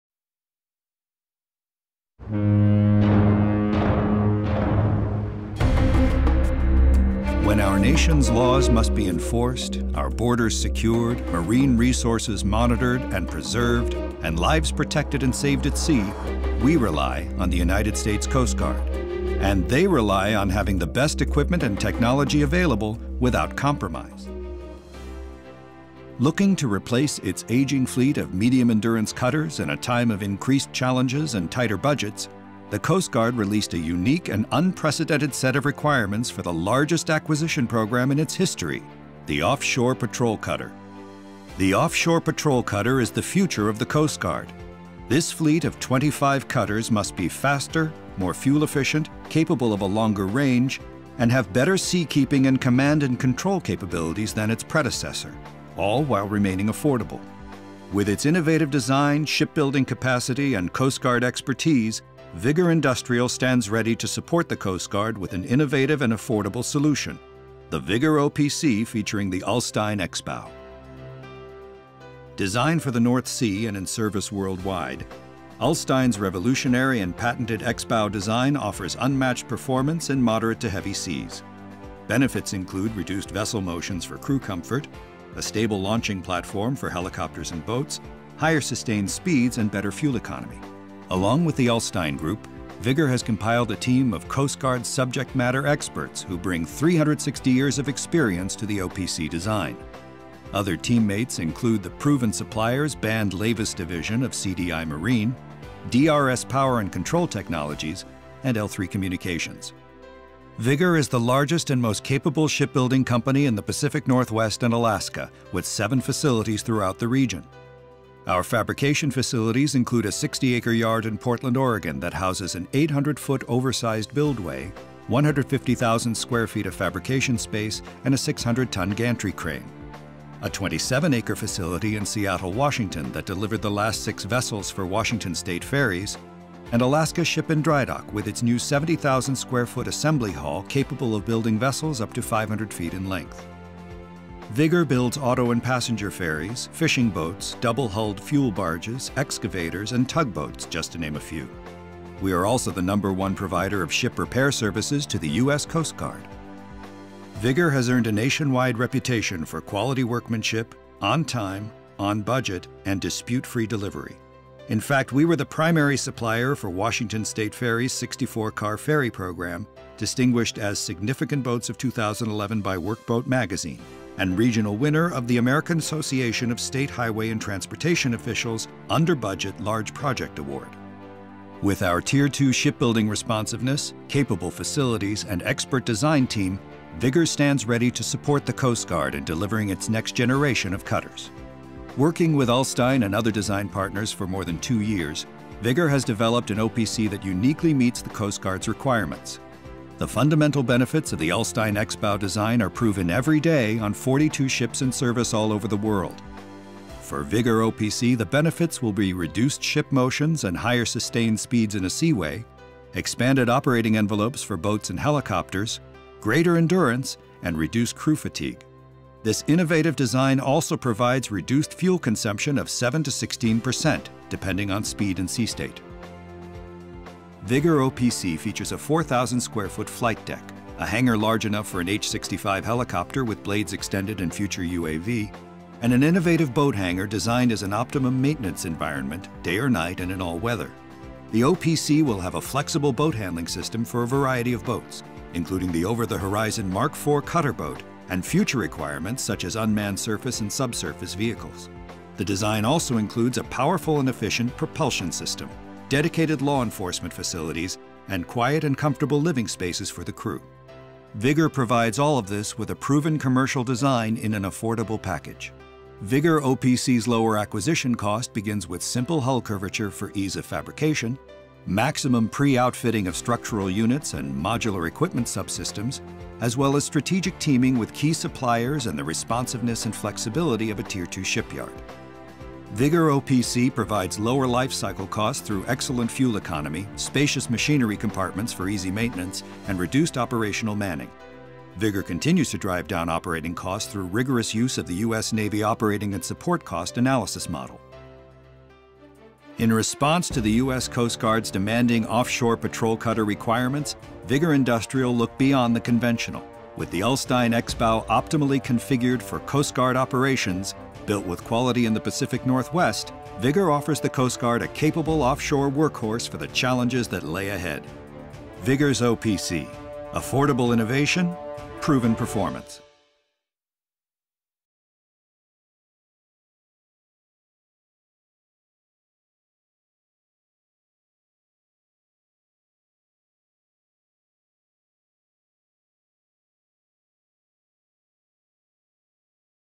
Vigor Offshore Patrol Cutter with Ulstein X-BOW® - USCG
In response to the U.S. Coast Guard's demanding Offshore Patrol Cutter requirements, Vigor Industrial looked beyond the conventional. With the Ulstein X-BOW®, the Vigor OPC delivers unmatched seakeeping and endurance. The Ulstein X-BOW is an inverted bow designed by Ulstein Group to improve handling in rough sea, and to lower fuel consumption by causing less hydrodynamic drag.